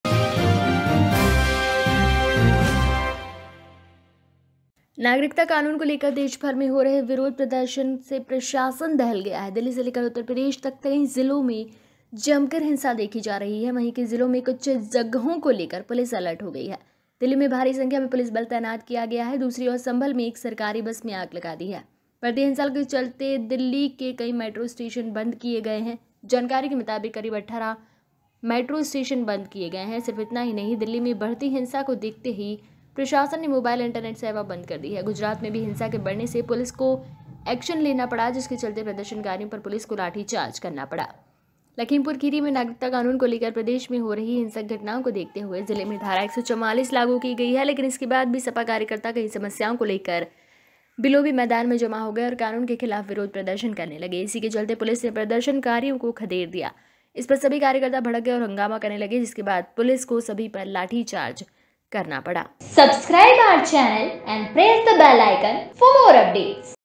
CAA Protest : नागरिकता कानून के खिलाफ पूरे देश में उपद्रव आगजनी | Citizenship Ammendment Act
कहां पर क्या-क्या हुआ |